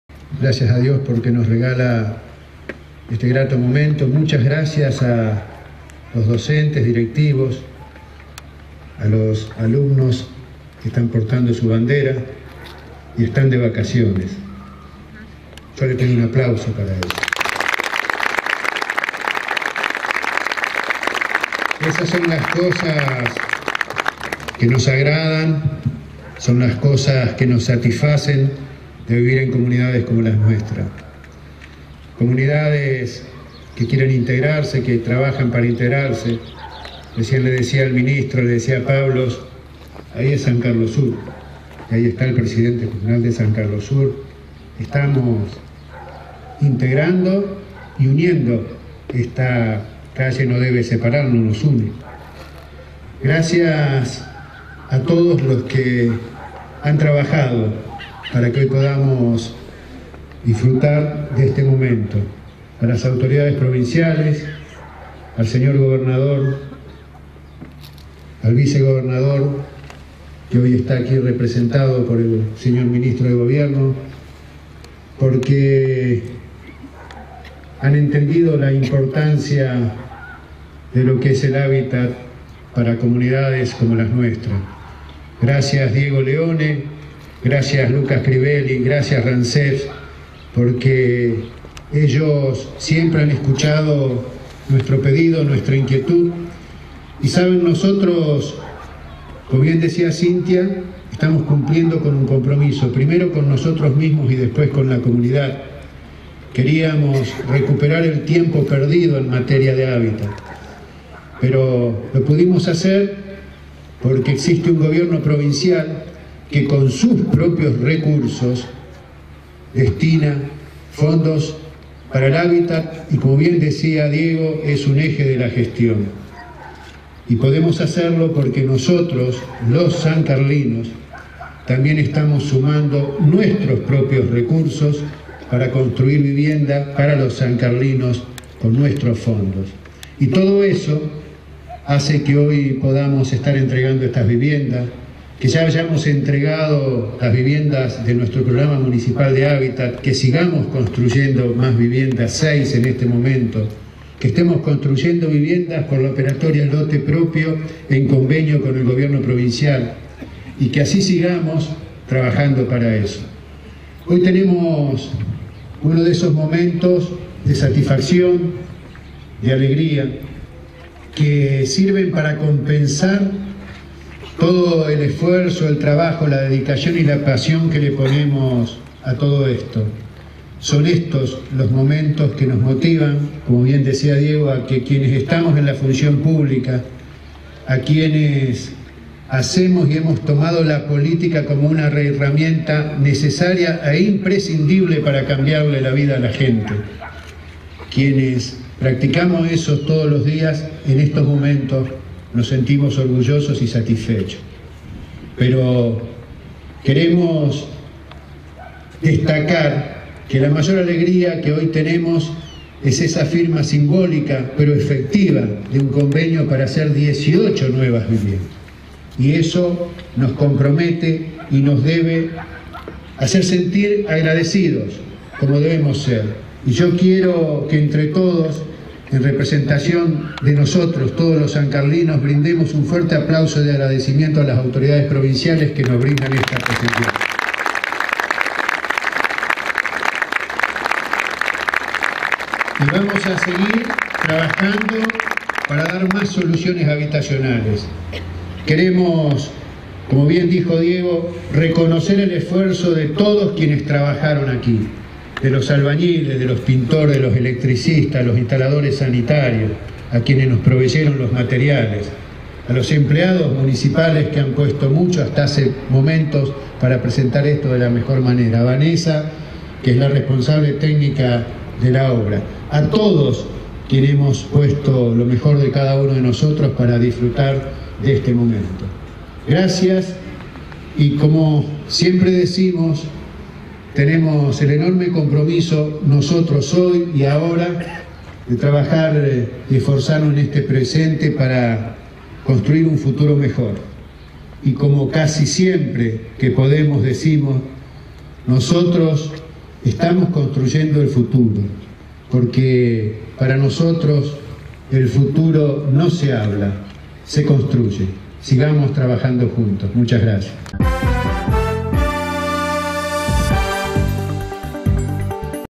Int. Jorge Placenzotti - Jefe de Gobierno de San Carlos Centro
Entrega de 16 viviendas en el extremo sur de la ciudad